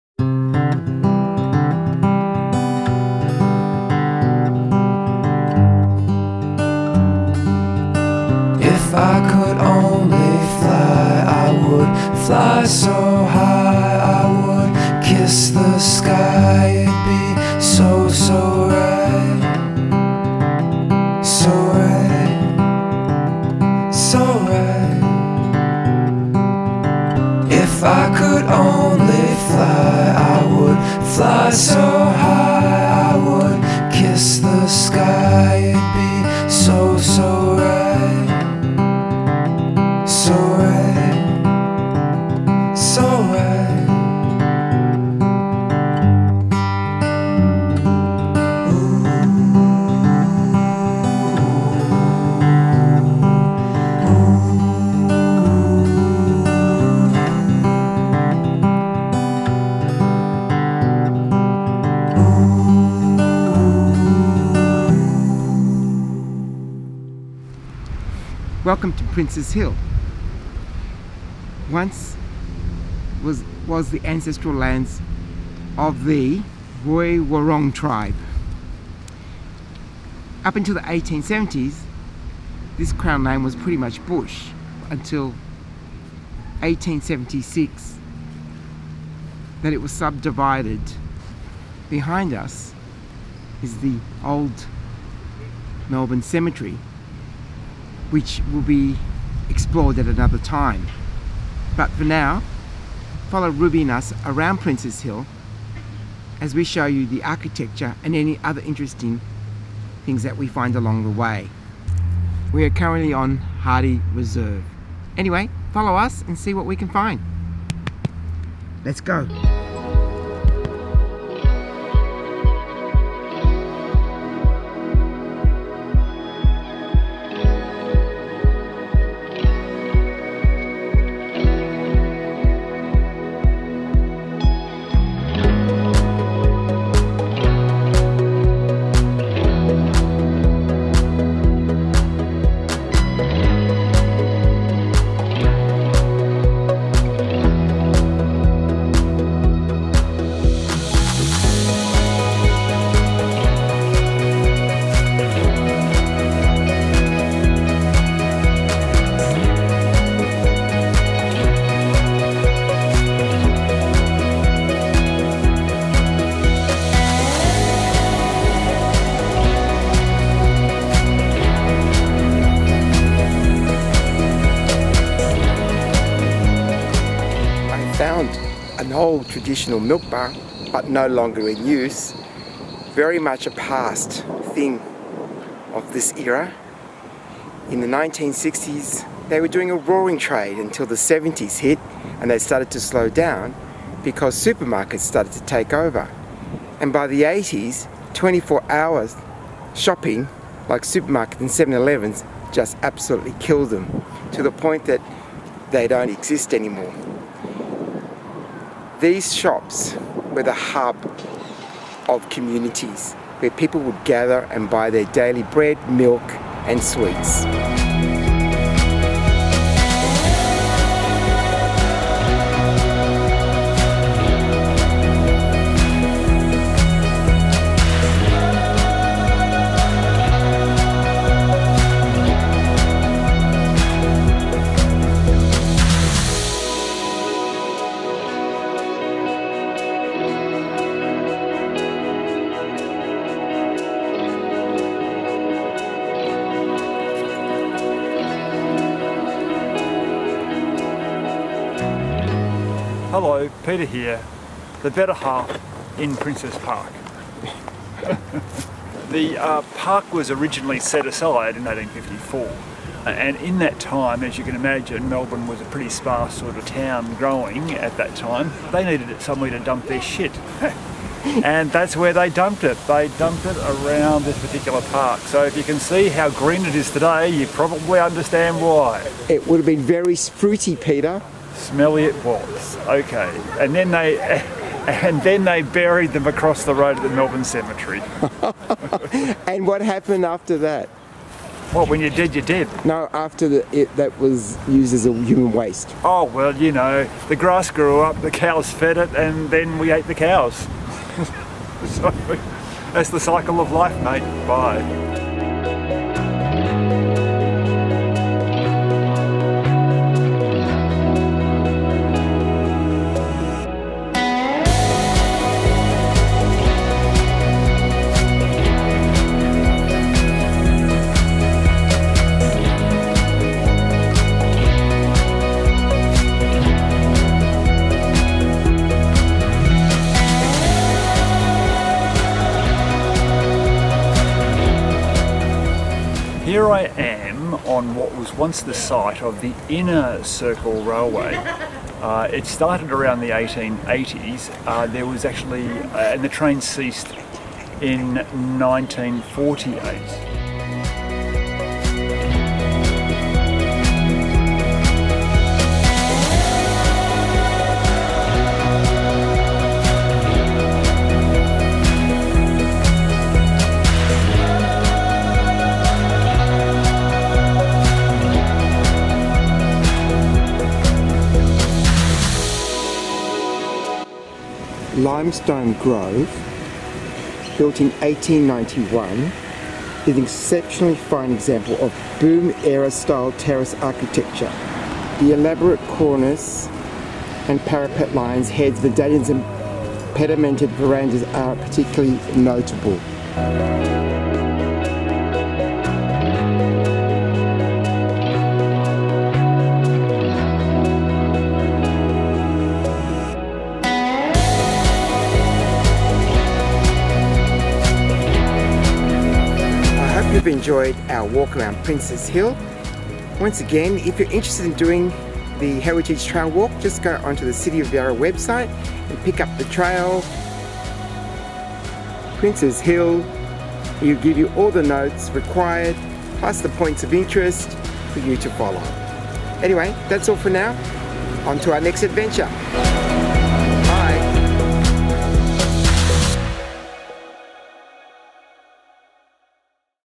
PRiNCES HILL HERiTAGE WALK ~ Victoria
Peter and Joseph take a heritage walk around Princes Hill and discover some of the mix of historical architecture.
The main period of development took place between 1870 and 1910. Lovely open street layouts and minimal commercial development. Come and explore some of the Melbourne's beautiful architecture.

Princes Hill is an inner suburb of Melbourne, Victoria, Australia, 3 km north of Melbourne's Central Business District. Its local government area is the City of Yarra.
Princes Hill was named for the Prince of Wales, similarly to neighbouring Carlton.

I get my music from ARTLIST, but clicking on the link you can support me.